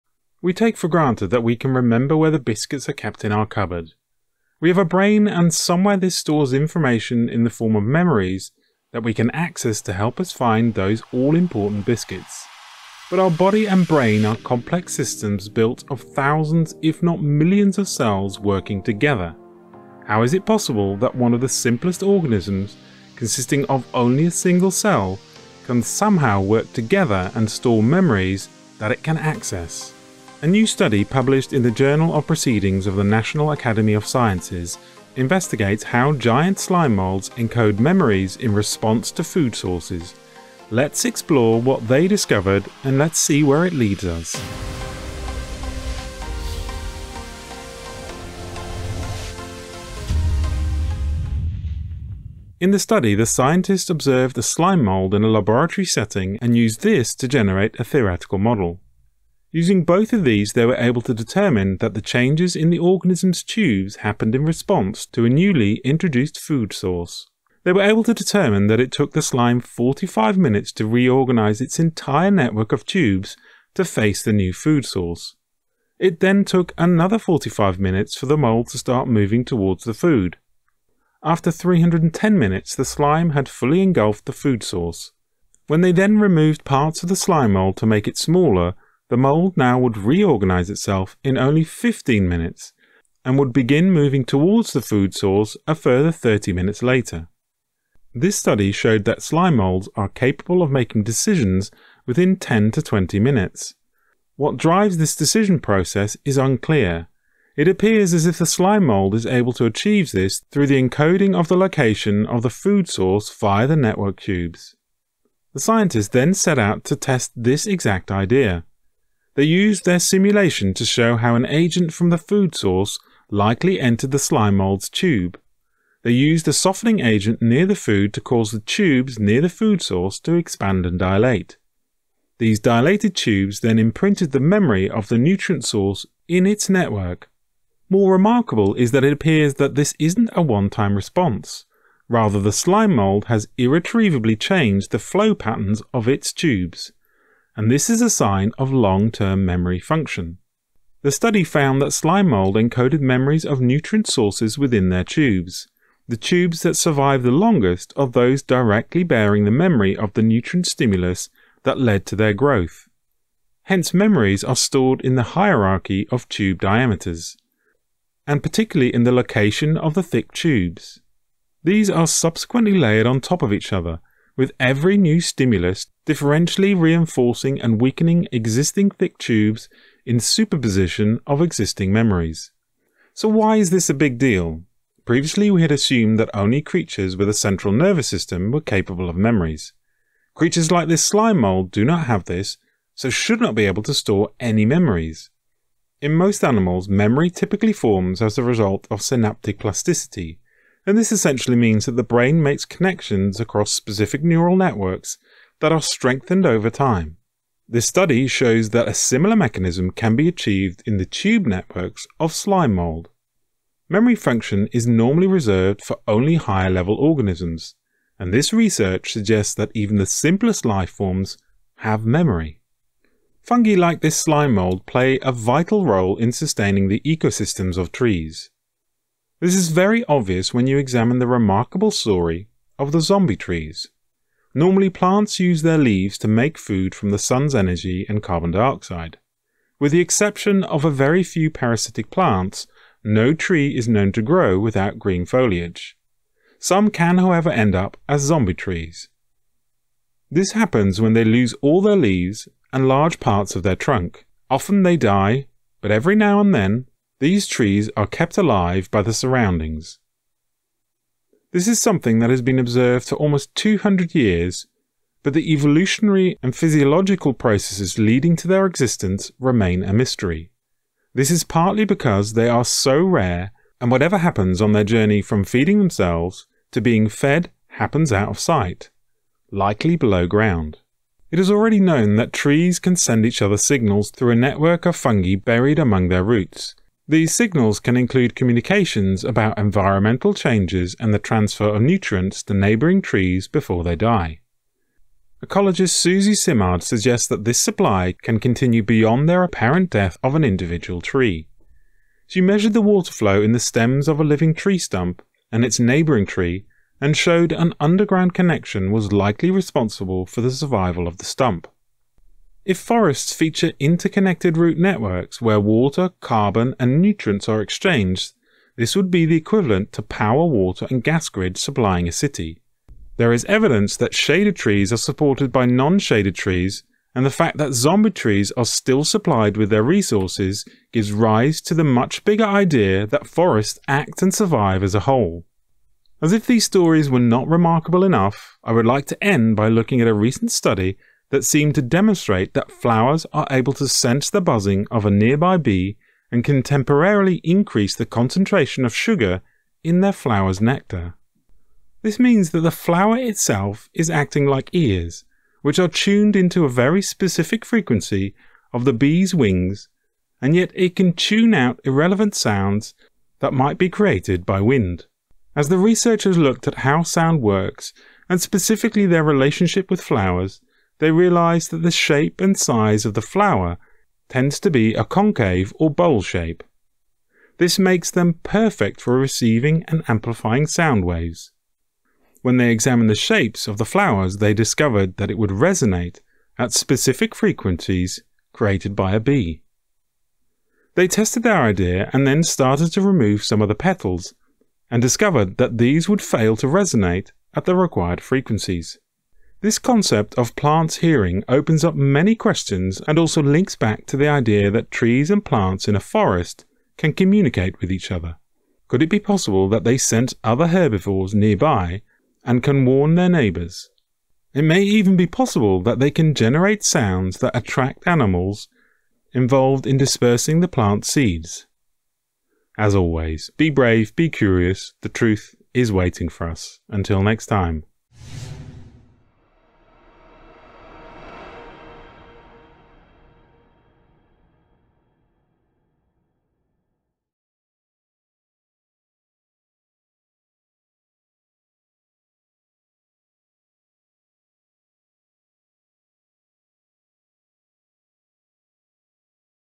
Single Cell Organisms with Long Term Memory Challenges our Understanding of Memory and Intelligence
We take for granted that we can remember where the biscuits are kept in our cupboard.  We have a brain and somewhere this stores information in the form of memories that we can access to help us find those all important biscuits.

But our body and brain are complex systems built of thousands if not millions of cells working together.  How is it possible that one of the simplest organisms consisting of only a single cell can somehow work together and store memories that it can access?

A new study published in the journal Proceedings of the National Academy of sciences, investigates how giant slime mold encode memories in response to food sources.

We will also look at the remarkable case of zombie trees and see once again how Fungi play a critical role in providing a network that connects the trees together.

Lastly we will look at a very intriguing new study that seems to demonstrate that flowers can hear the sound of approaching bees and adjust the nectar in the flowers.

00:00 Intro
00:52 Giant Slime Mould with Memory
03:43 Zombie Trees
05:36 Flowers that can Hear